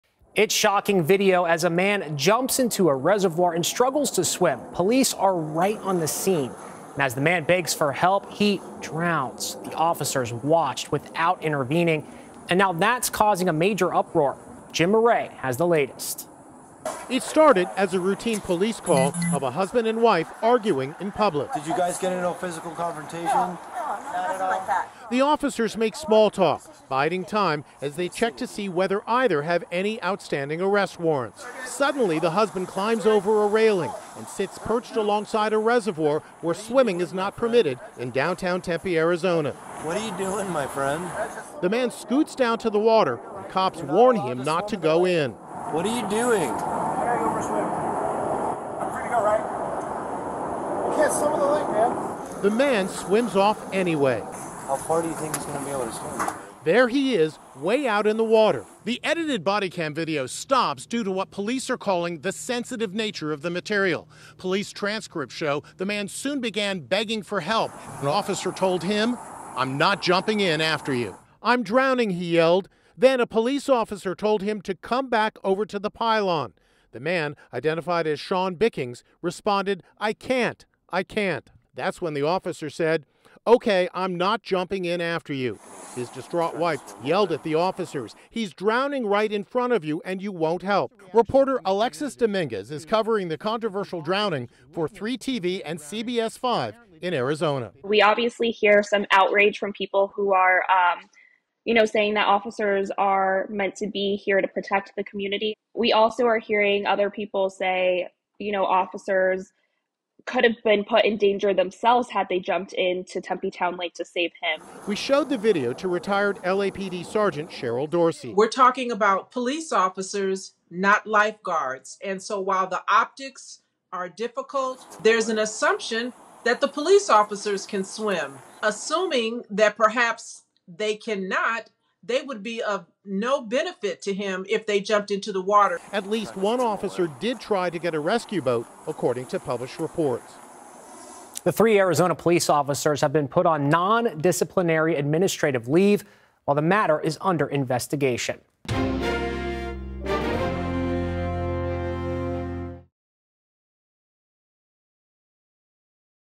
Cops Allegedly Refuse to Rescue Man Who Drowned
A man is dead after drowning in a reservoir in Tempe, Arizona. Initially, police responded to reports of the man and his wife arguing. The officers made small talk with the couple as they checked to see if either had outstanding arrest warrants. Suddenly, the man climbed over a railing and perched himself over the reservoir. The officers warned him not to try to swim in it, but he ignored them. Soon, the man was begging for help.